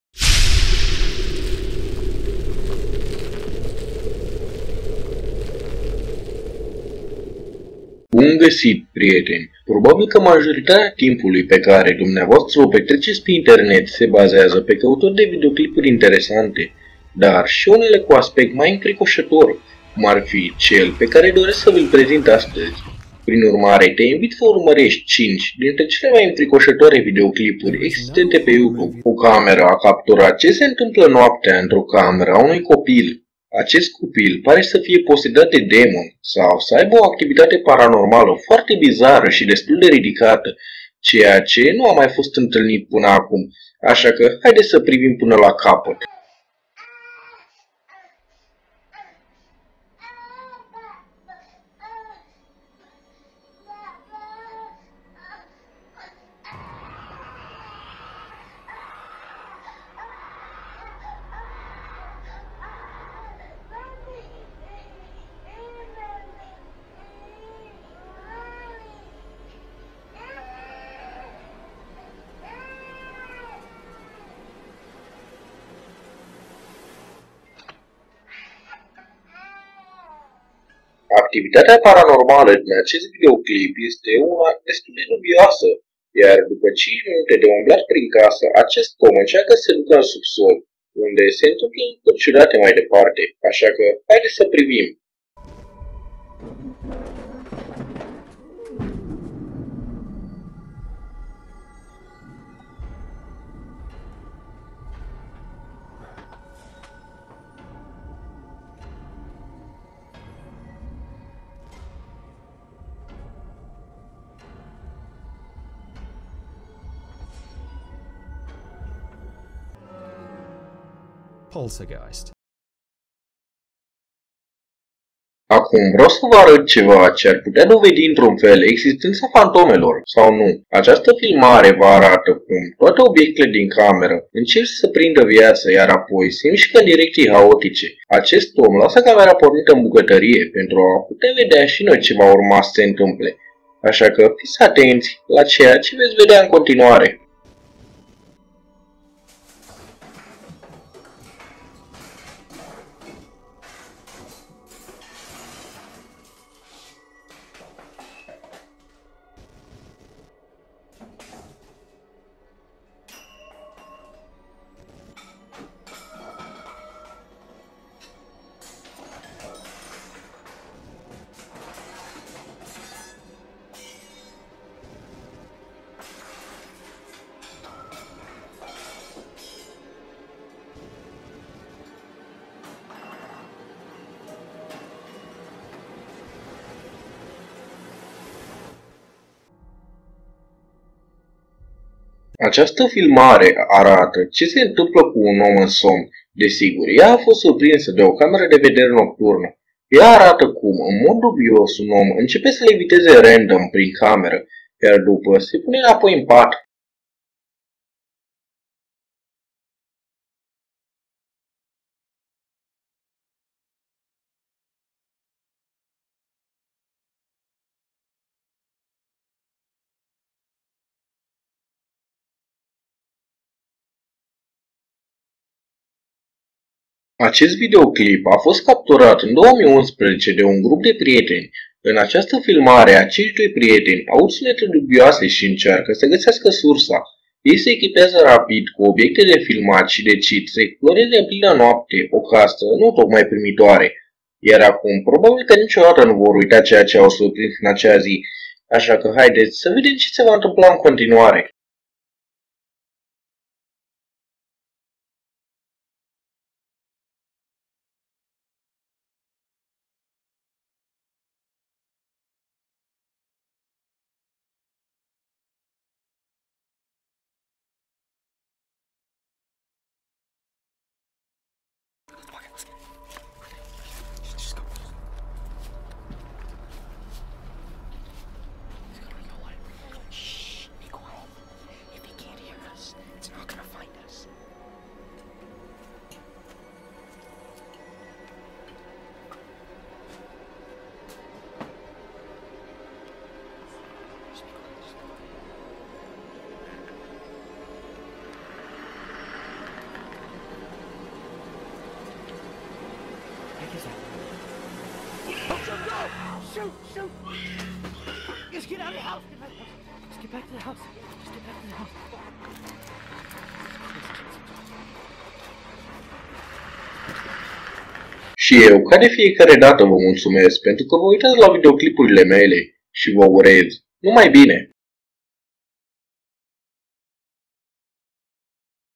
5 Videoclipuri Infricosatoare(care te vor tine treaz)
Bun gasit prieteni,probabil ca majoritatea timpului pe care dumneavoastra o petreceti pe internet se bazeaza pe cautari de videoclipuri interesante,dar si unele cu un aspect mai infricosator,cum ar fi cel pe care doresc sa vil prezint azi.Prin urmare,te invit sa urmaresti 5 dintre cele mai infricosatoare videoclipuri existente pe youtube.

Daca ti-a placut nu uita sa lasi un like,un share si daca dorest sa te abonezi.